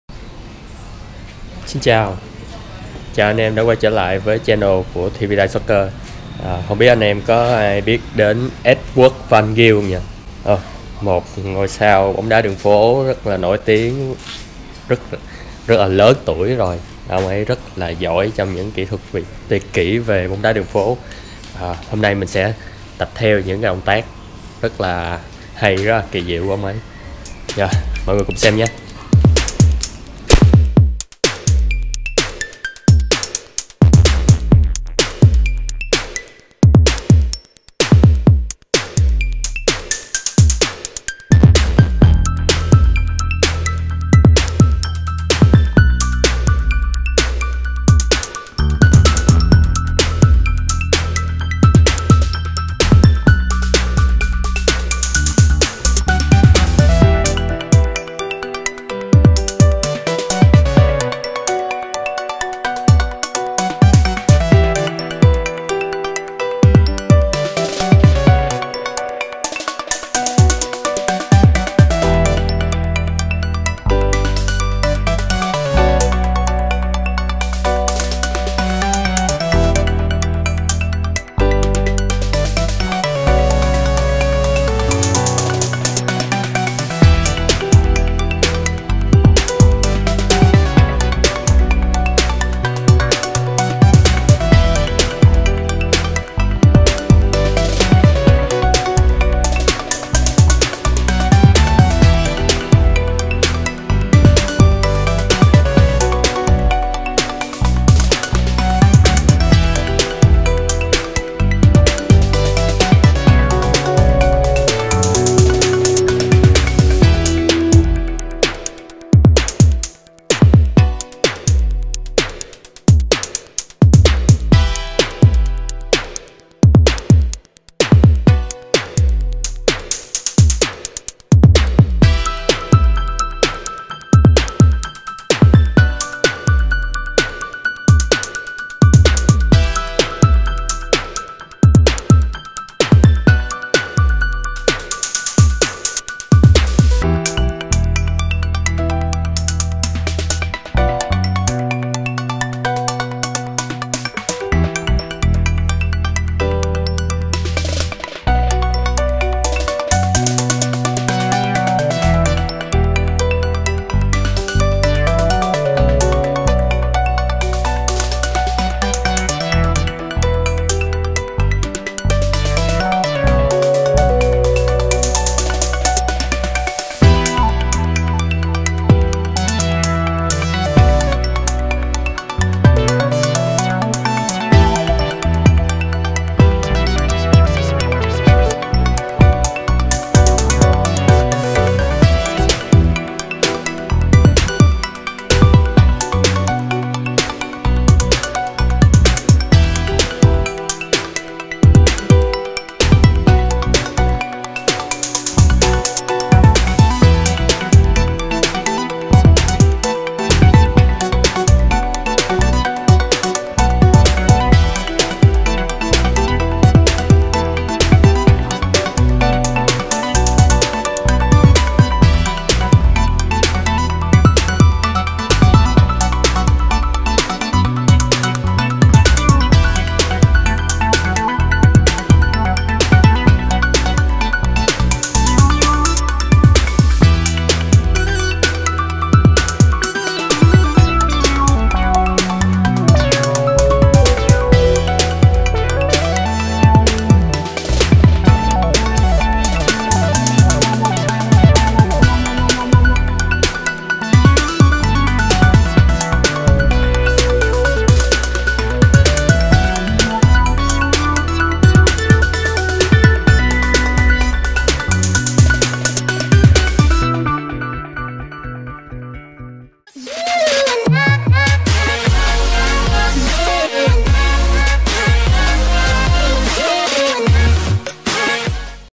Tập luyện 9 kỹ năng bóng đá đường phố cực hay của Edward Van Gils
Hôm nay Thy tập luyện một số kỹ thuật bóng đá đường phố cực hay của God-Father of Street Football. Anh em có thể xem và tập theo nhé.

Nếu anh em thấy hay thì like và share đi nào.

Tập luyện 9 kỹ năng bóng đá đường phố cực hay như Edward Van Gil - Một sản phẩm từ Thy Freestyle Soccer